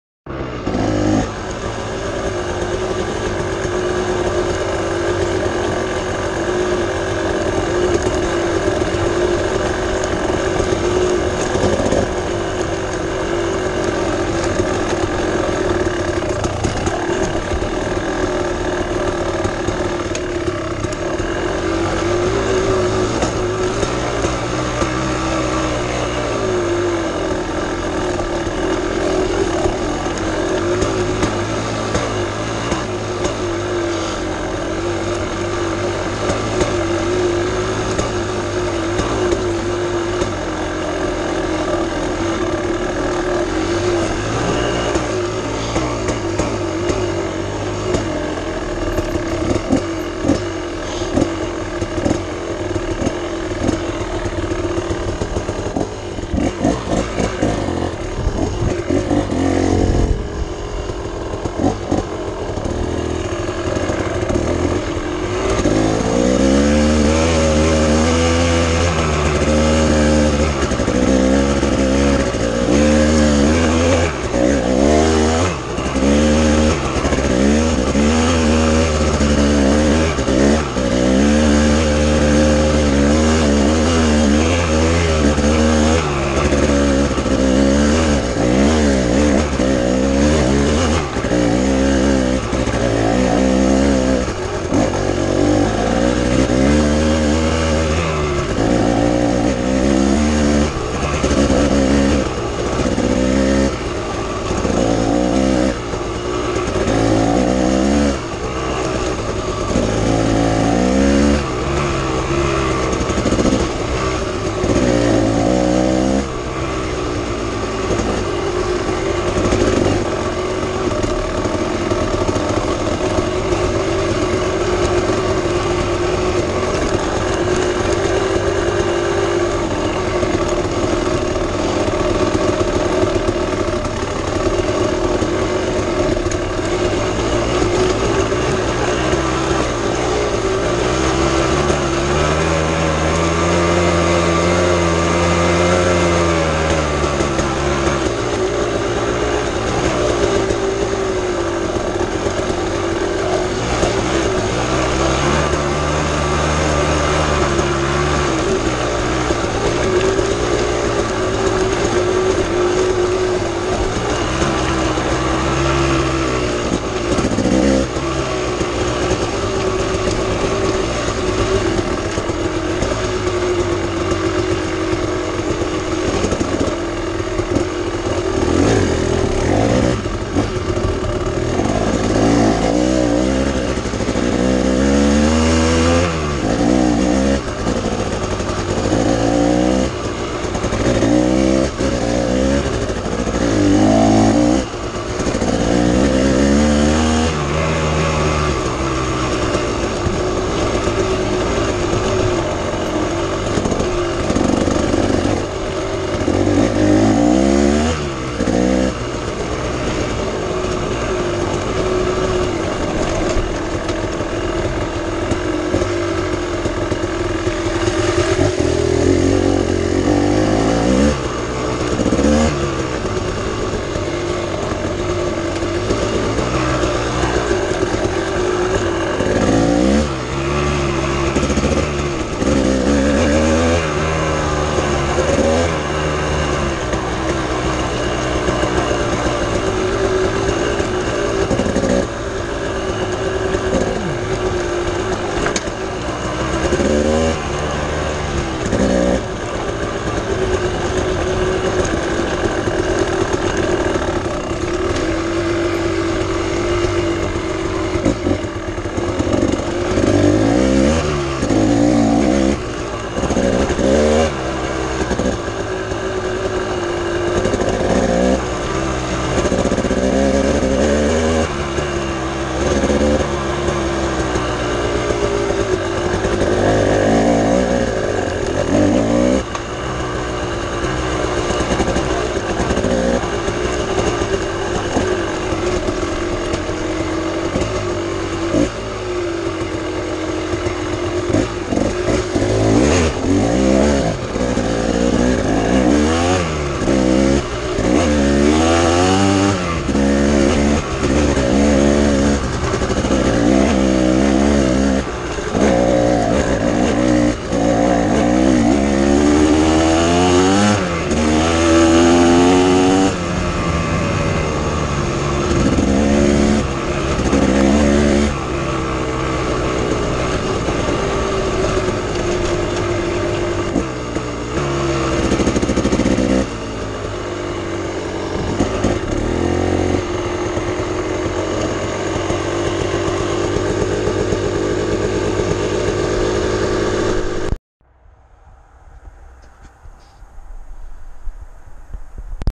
KX220 Charleston, ME Trail Near Golf Course
Please excuse the fuzzy images. It was a wet fogy day and the camera lens had water on it. I'll try to shoot this section of trail again some time when it's not so wet. This is a section of ATV trail in Charleston, ME near where Route 15 intersects with the School Rd.